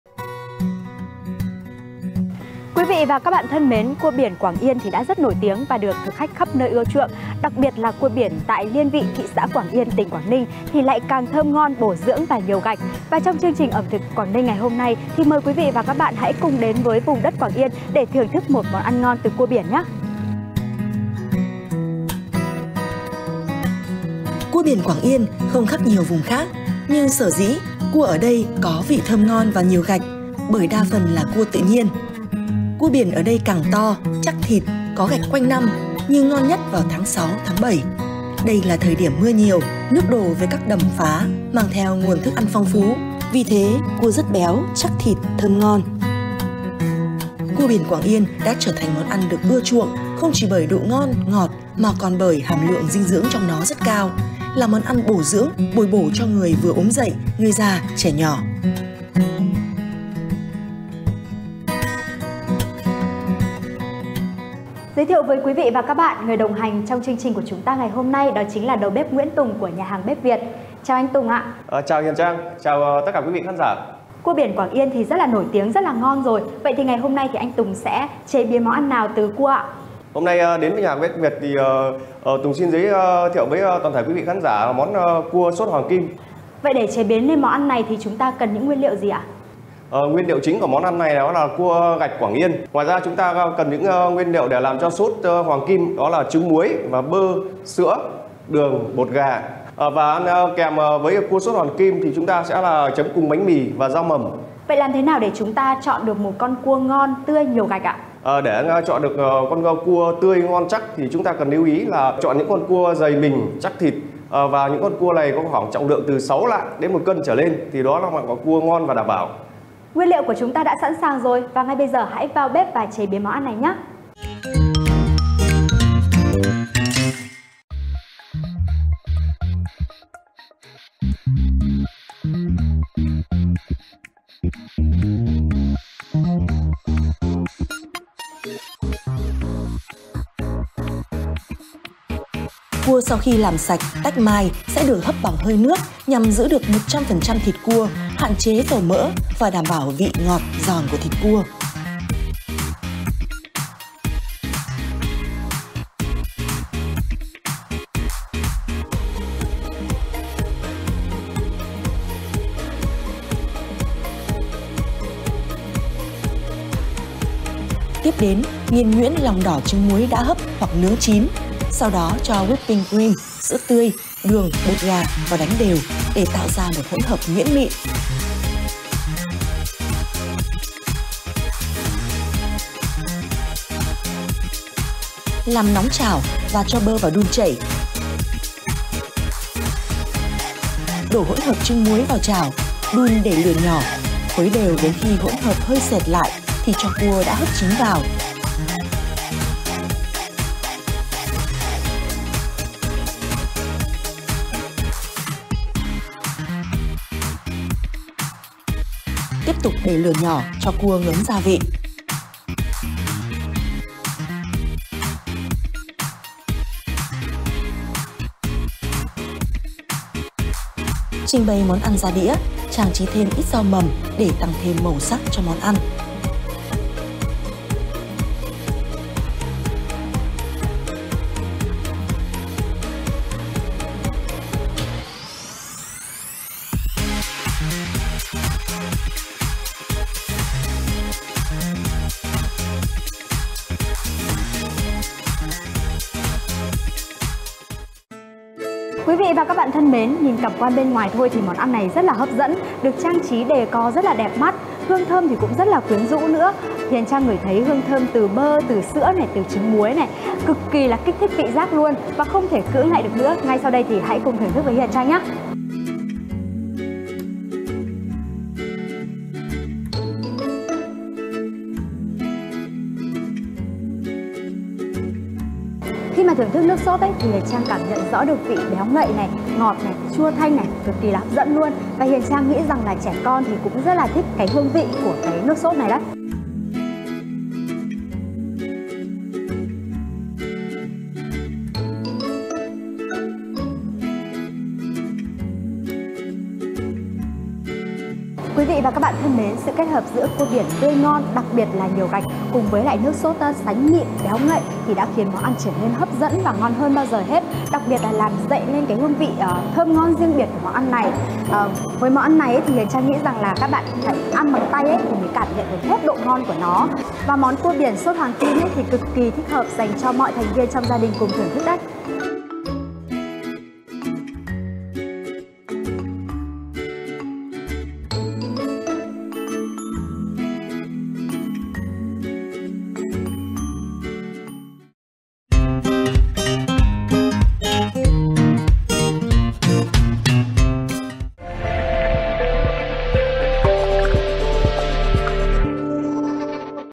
Cua biển Quảng Yên xốt hoàng kim
Cua biển Quảng Yên xưa nay vốn nổi tiếng và được thực khách từ khắp nơi ưa chuộng bởi đặc biệt thơm ngon, bổ dưỡng và nhiều gạch. Với món "Cua biển Quảng Yên xốt hoàng kim", sự kết hợp giữa thịt cua biển tươi, chắc mẩy hoà quyện với nước xốt mang điểm nhấn là vị mặn, chua, cay, ngọt vừa phải chắc chắn là điều thực khách nào cũng mong muốn được thưởng thức.